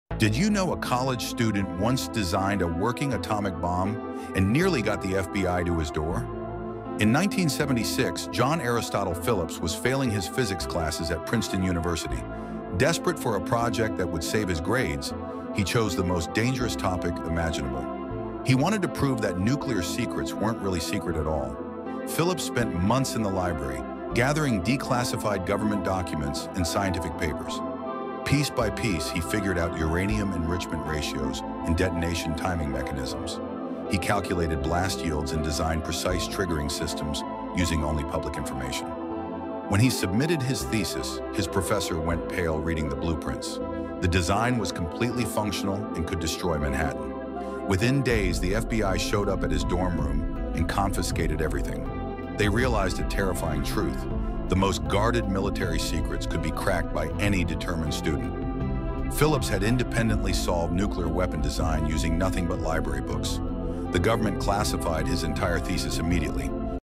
College Student Designed Working Nuclear Bomb & FBI Showed Up at His Door
Student nearly got arrested for this crazy project